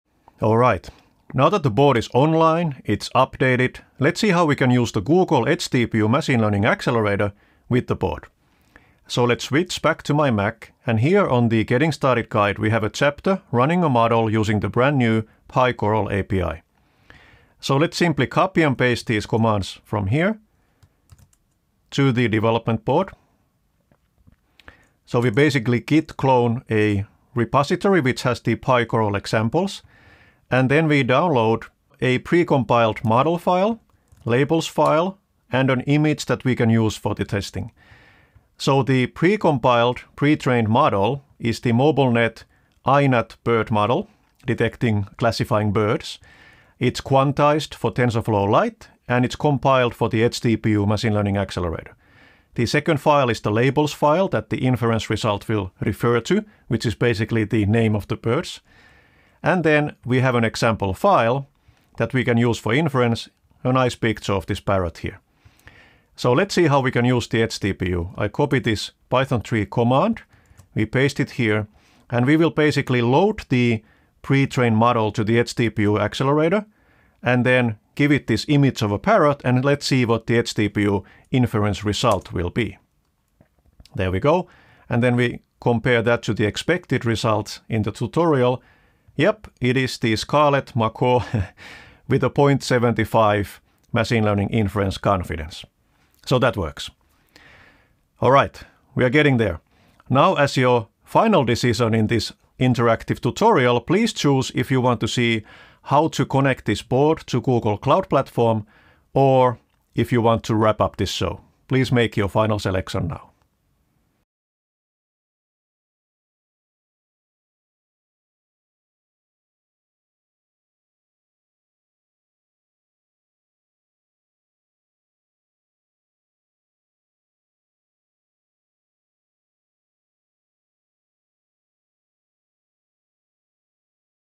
Mini - Run ML model
This is a segment of the interactive Google Cloud Level Up tutorial - Getting Started with Coral Dev Board Mini